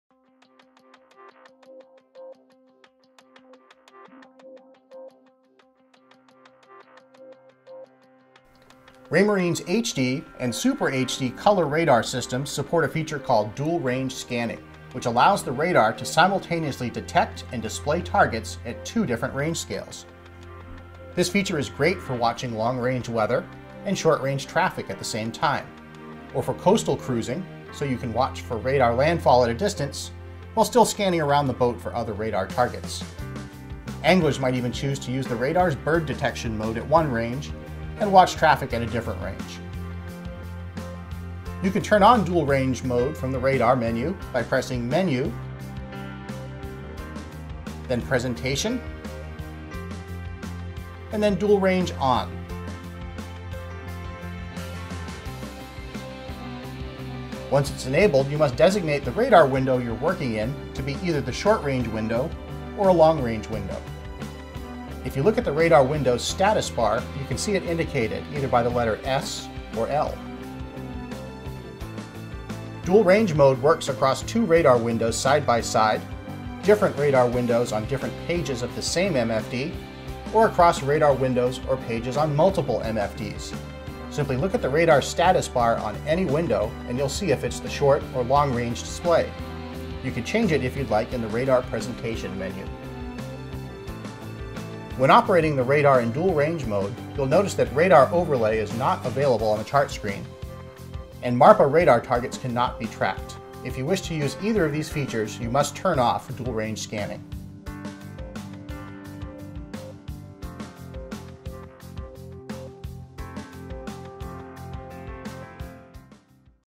LightHouse II Tips and Tricks Dual Range Radar-iboats.com
Raymarine HD and SuperHD radar systems support  dual range scanning.  This allows the radar to dectect and display targets simultaneously.